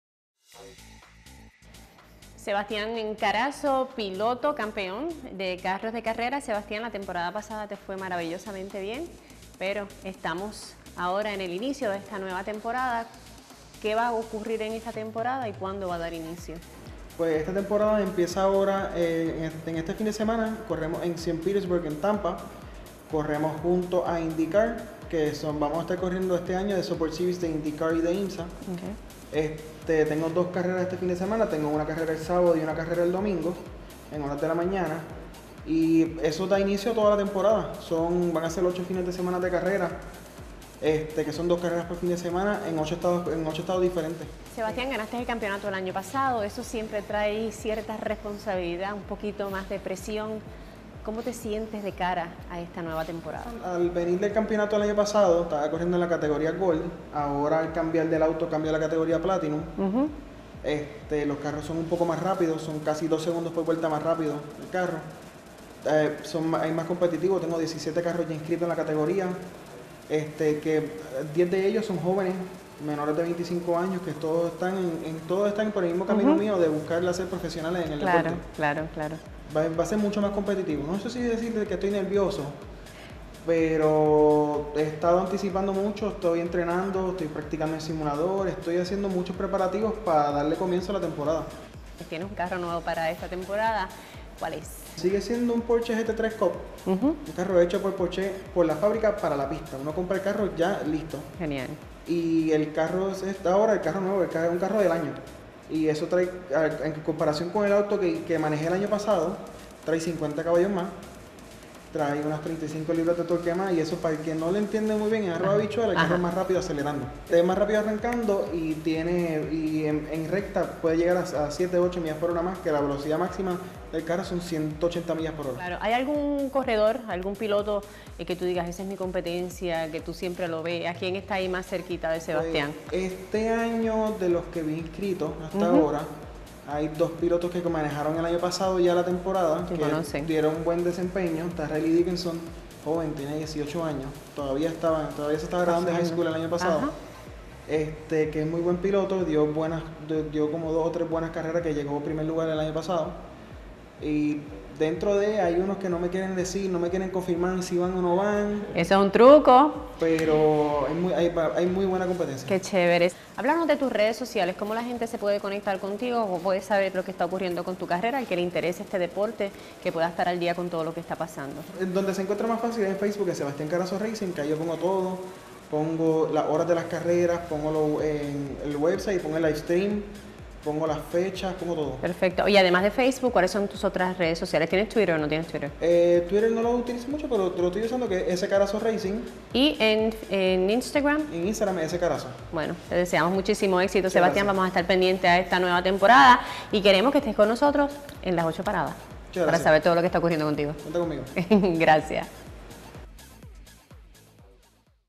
Piloto puertorriqueño Sebastián Carazo listo para nueva temporada de carreras
El puertorriqueño Sebastián Carazo, piloto campeón de la división gold del Porsche GT3 Cup Challenge USA y recipiente del FIA Americas Award 2019, como mejor piloto de Puerto Rico, está listo para enfrentar una nueva temporada de carreras en el 2020. A solos días de retomar el volante el próximo 14 de marzo en St. Petersburg, Florida, la compañera Natalia Meléndez dialogó con él en torno a sus nuevos retos, su preparación y las expectativas sobre las competencias que se avecinan.